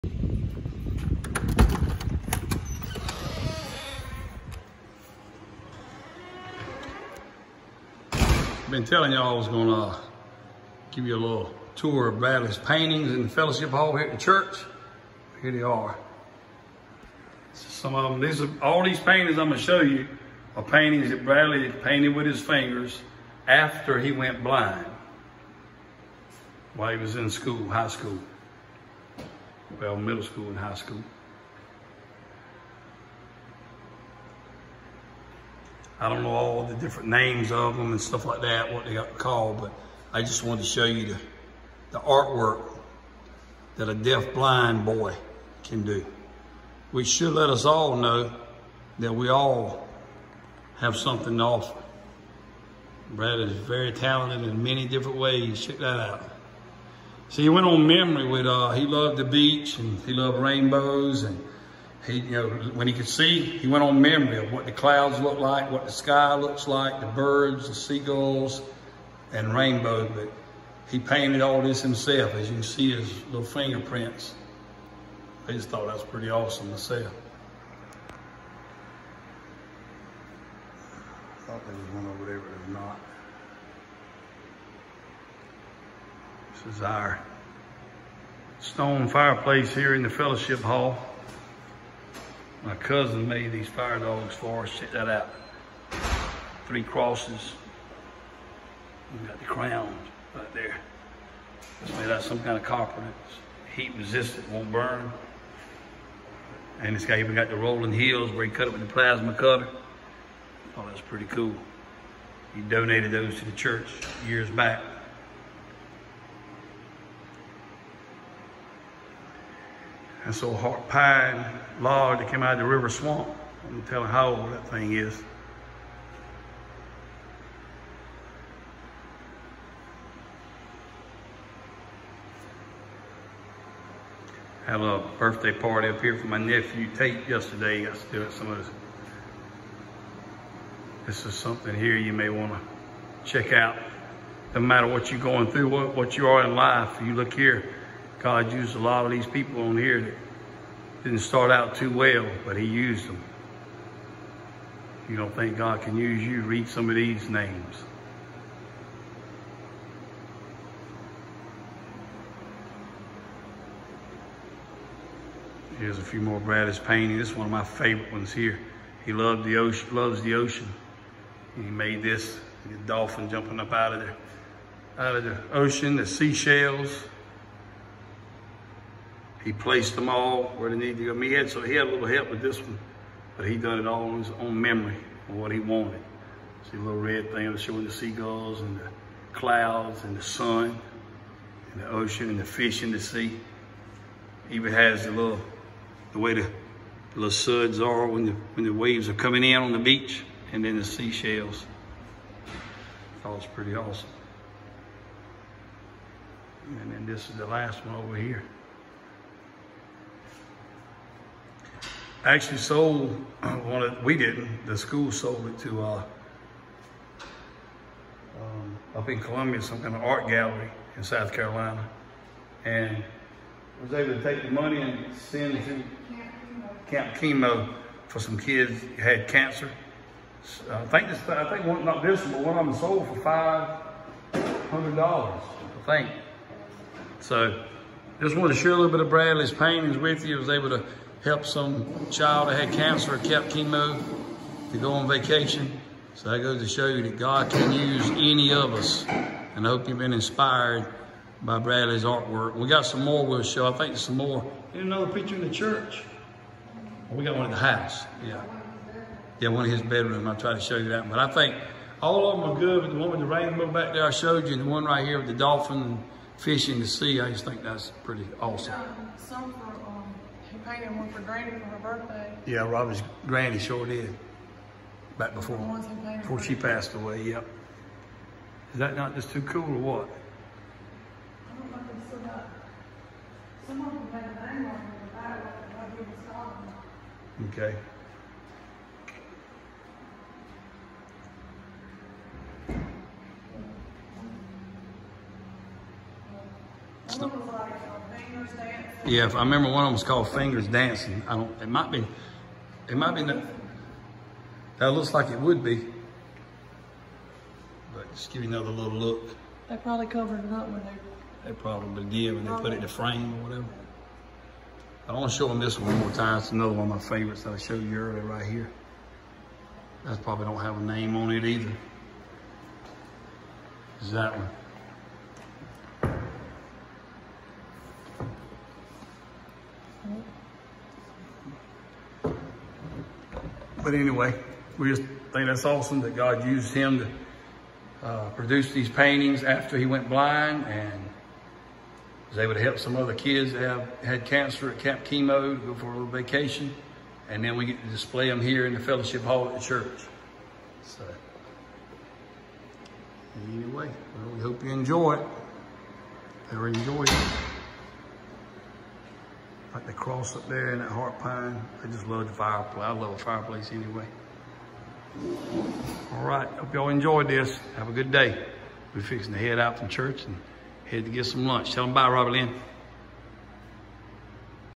FINGER PAINTINGS BY FARMER BRADLEY!! (DESPITE DEAFNESS & BLINDNESS)
Showing farmer Bradley’s Art work he finger painted while he was still in middle school and high school after he went blind at 7 years old. Bradley is legally deaf and blind. We proudly display these in our fellowship hall at our church (Flat Creek Freewill Baptist!!